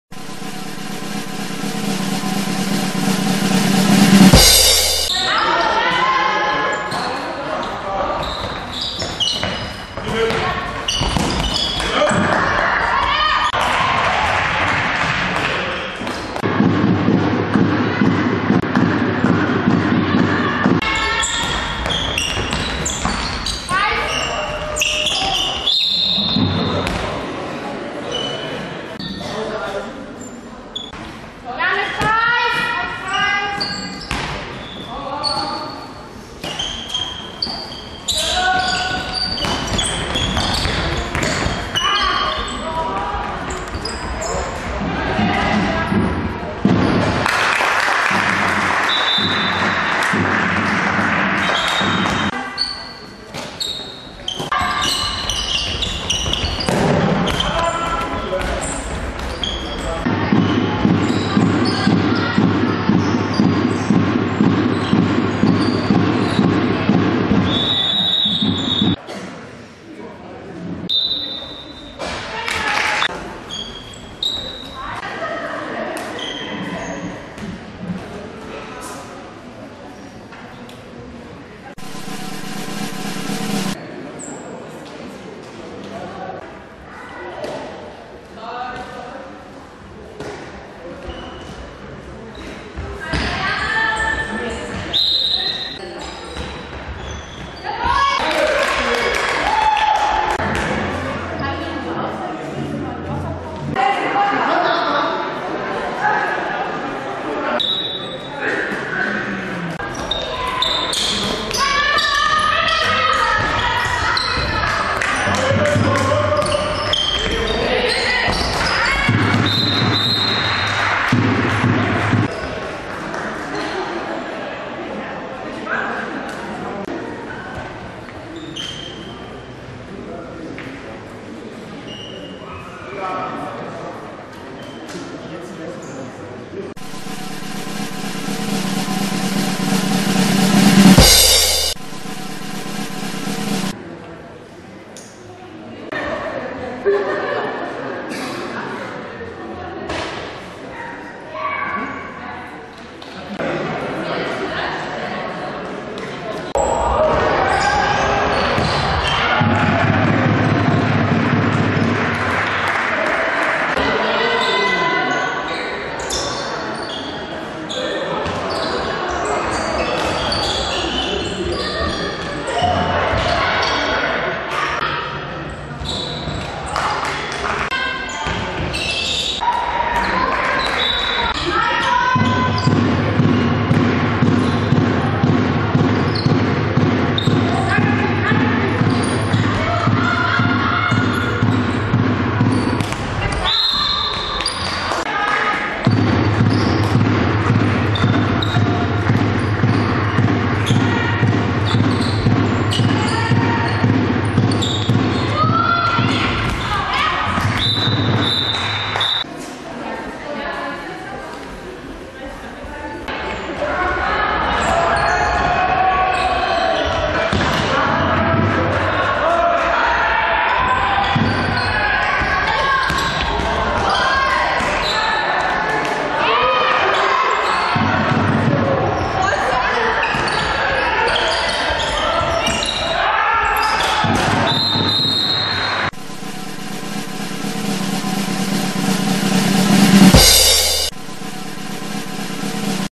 32:14 (15:6) SV GW Schwerin IV vs. HV Insel Usedom - 4.1.2015
HVMV Pokal Frauen Achtelfinale - 32:14 (15:6) SV GW Schwerin IV vs. HsV Insel Usedom - 4.1.2015